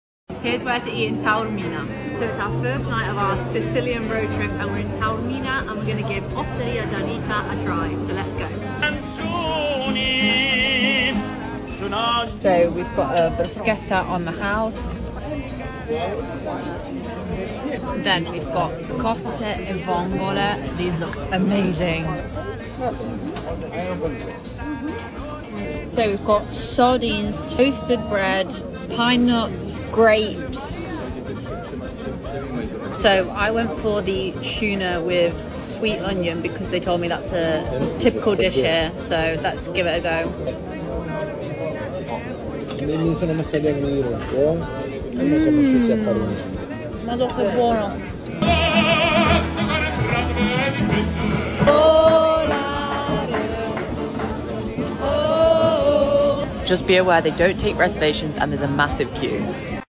Would definitely recommend eating here in Taormina, we had such a fun experience and it was very tas
Would definitely recommend eating here in Taormina, we had such a fun experience and it was very tasty! - sicilyroadtrip visitsicily sicilytrip taorminasicily taorminasicily❤️